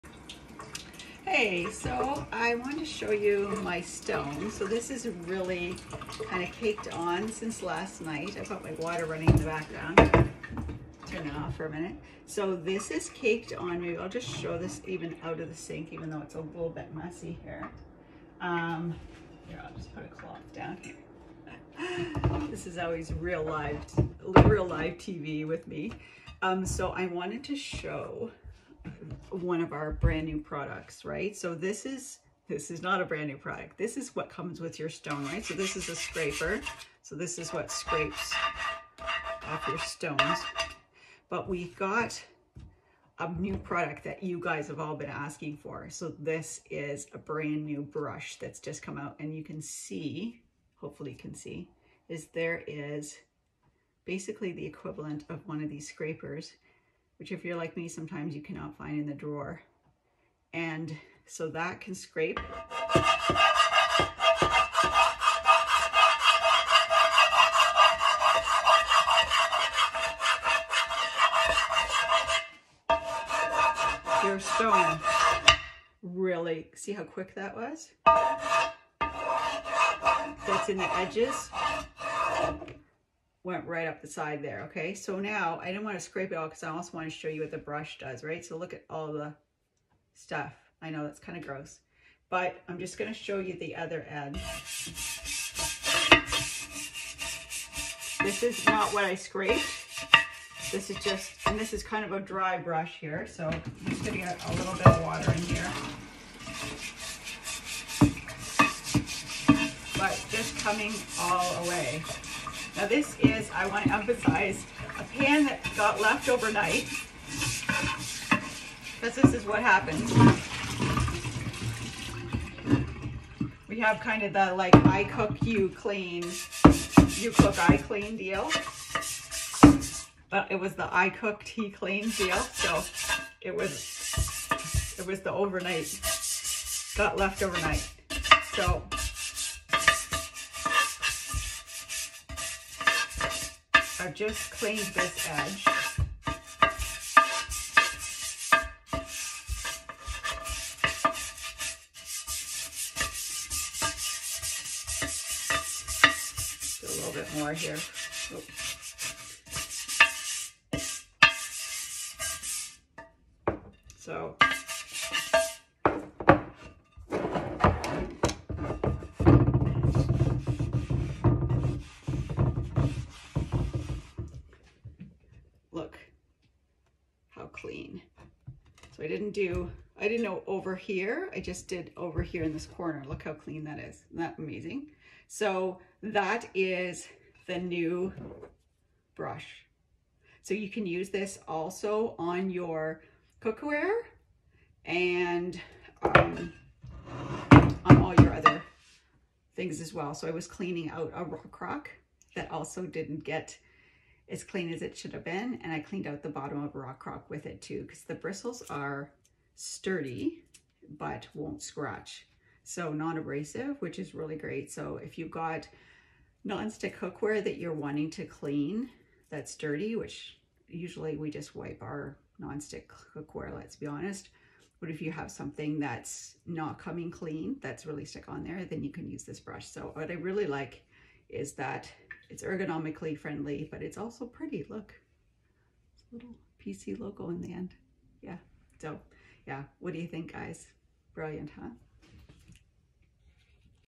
Cleaning up is one of the worst kitchen jobs so let’s get it done with one tool!
Have you been struggling to get your Air Fryer racks clean without soaking or the dishwasher?

Something soft enough for non-stick pans but tough enough for the baked on messes too! We’ve got it! We’ve brought back a ✨new✨Kitchen Brush that is up to the challenge!